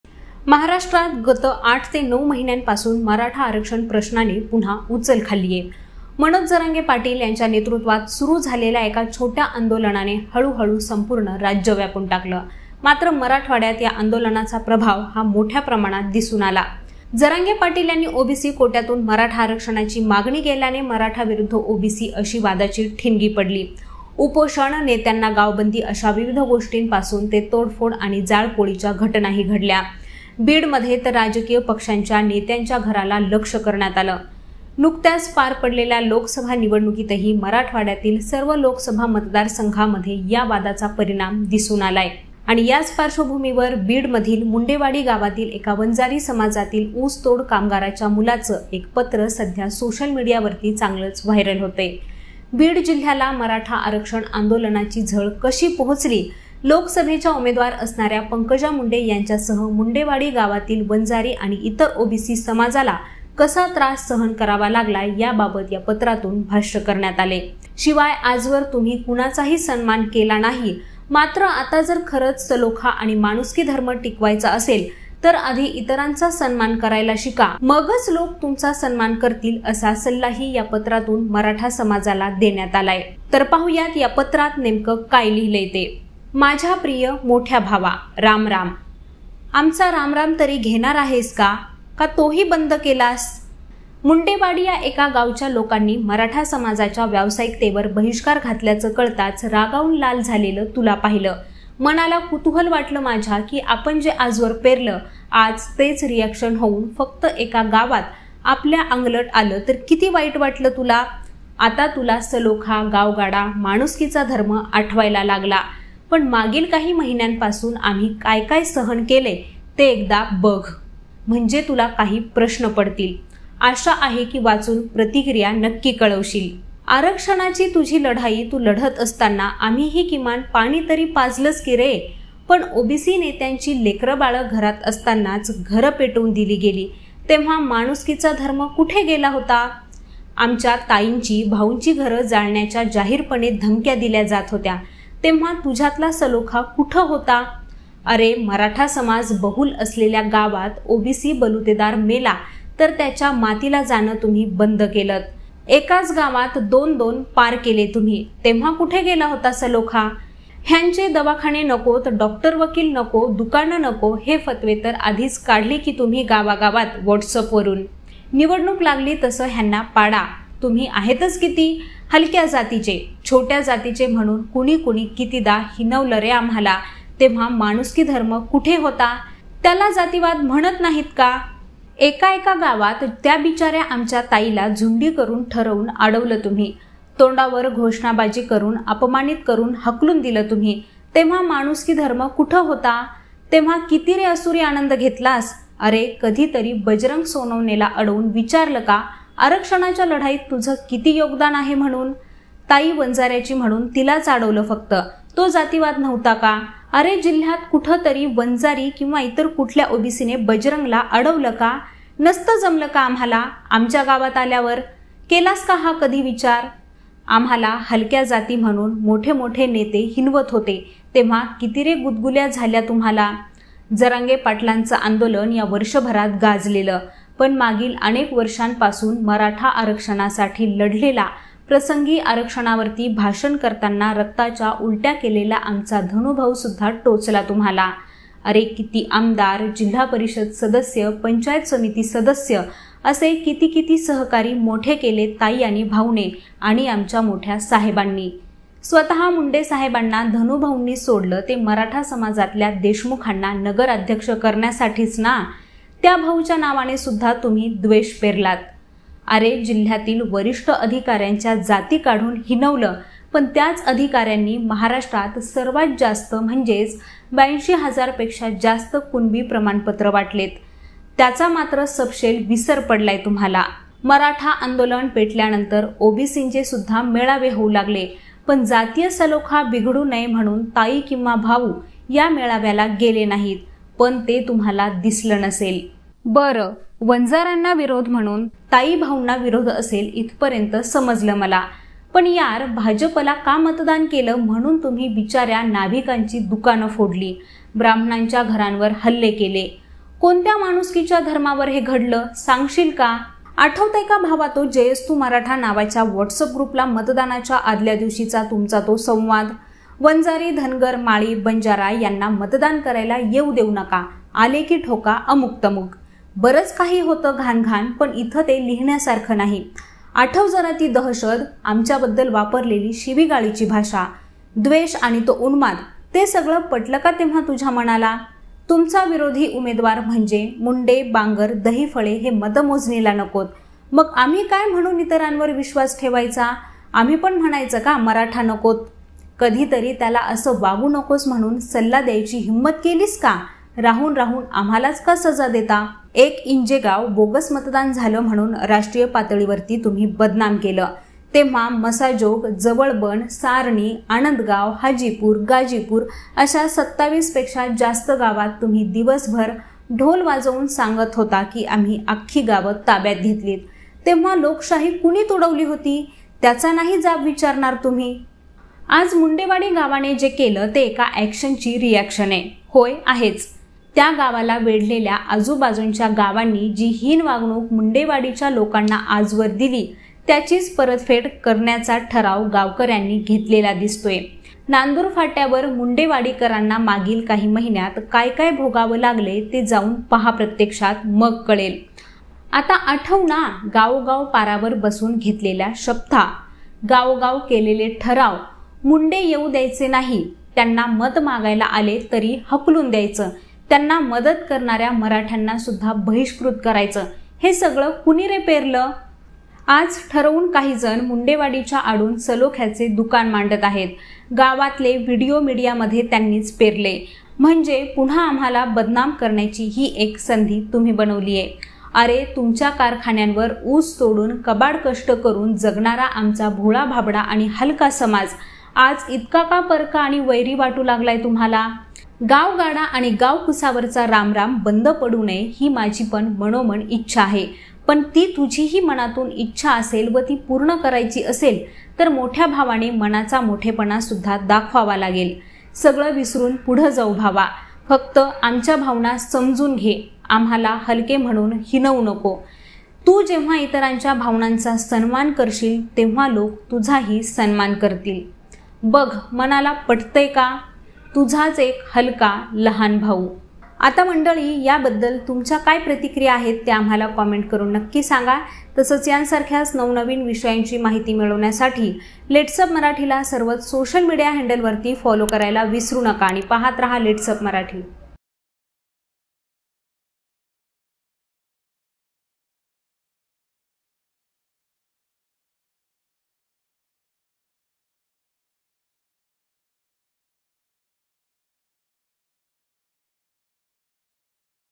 Mundewadi Viral letter : ऊसतोड कामगाराच्या मुलाचं मराठा समाजाला सल्ला दिलेलं पत्र | LetsUpp Marathi
बीडच्या मुंडेवाडी या गावातील एका वंजारी समाजाच्या ऊसतोड कामगाराच्या मुलाचे मराठा समाजाला सल्ला देणारे पत्र सोशल मीडियावर व्हायरल होत आहे. नेमका काय म्हटलं आहे या पत्रात जाणून घ्या सविस्तर...